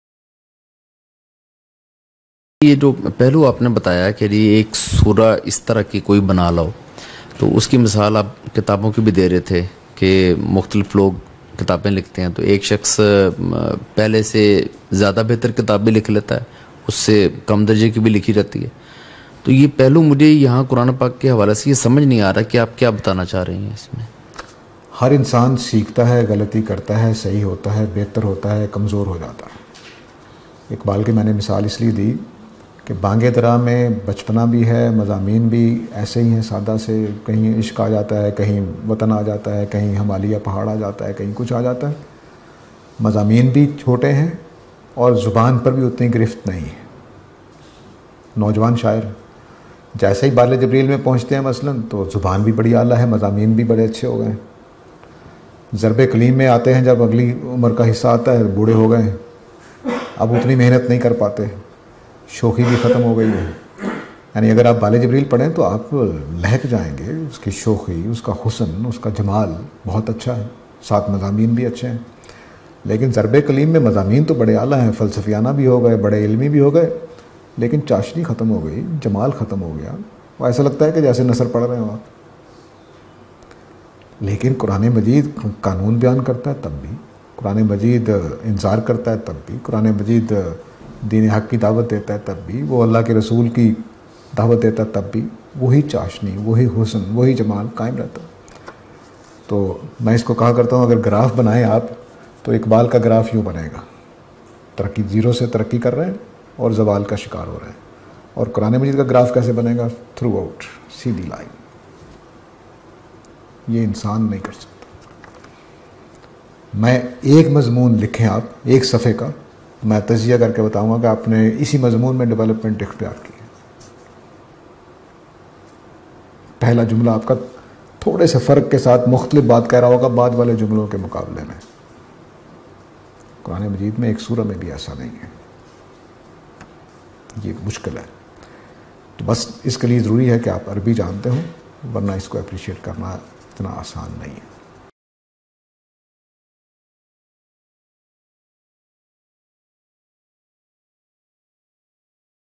Kya Quran mein adbi irtaqa nazar aata hai? | Dr. Sajid Hameed
Questions from Dars e Quran (30 May 2022)